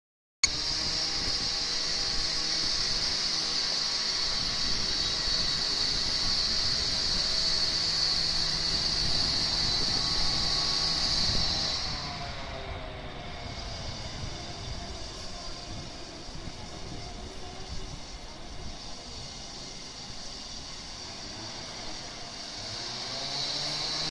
Przykładowy film nagrany Sony Xperia Z Ultra
Przykład jakości filmów nagrywanych smartfonem Sony Xperia Z Ultra.